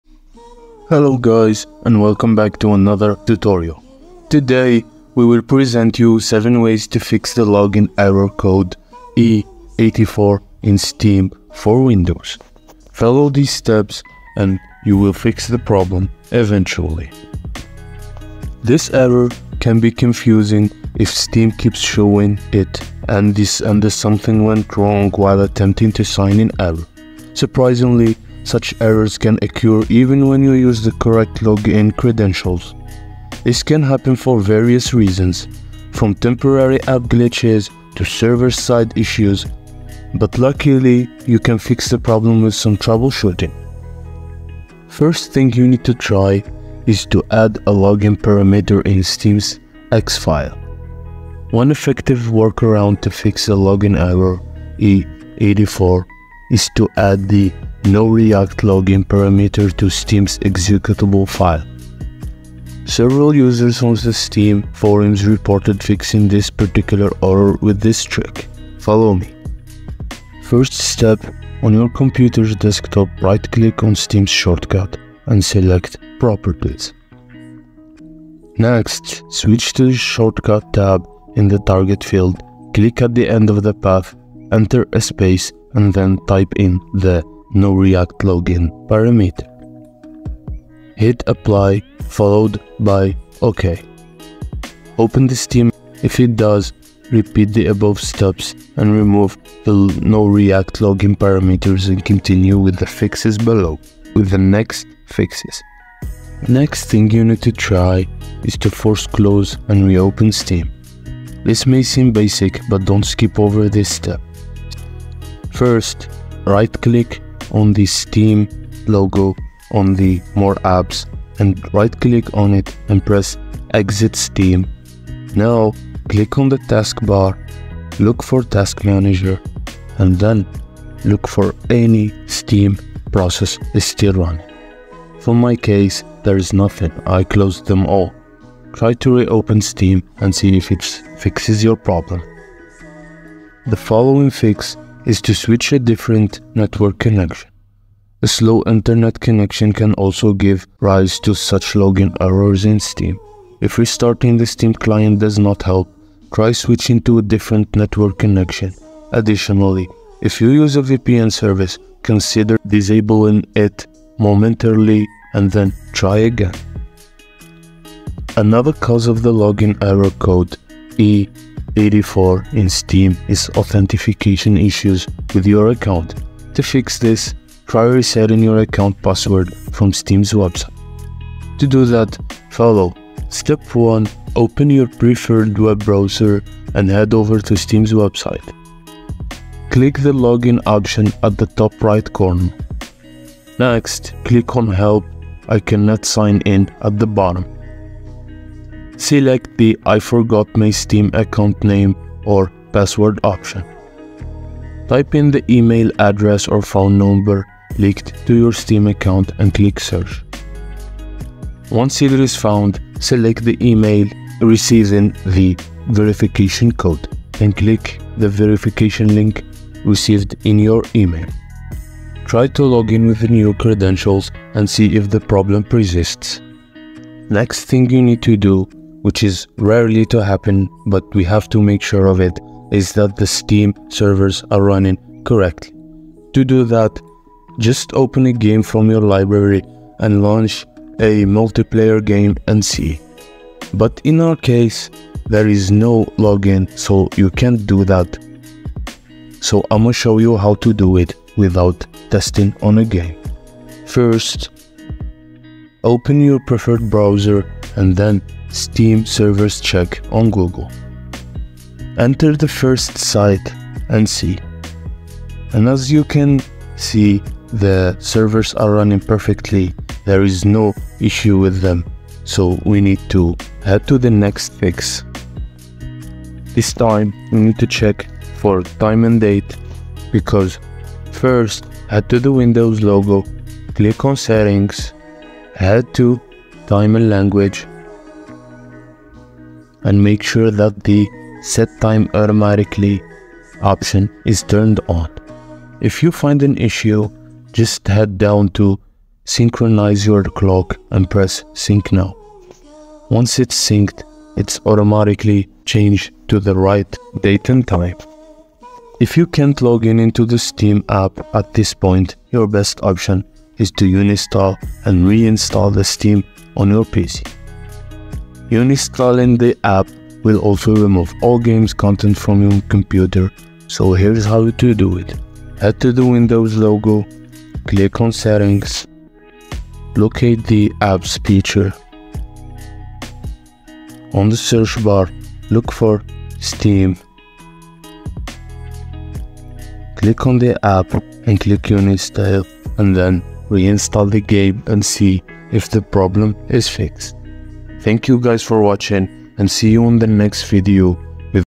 How To Fix Steam Error Code E84 (Solved 2025)
In this video I will show you how you can fix the steam error code e84 on your PC.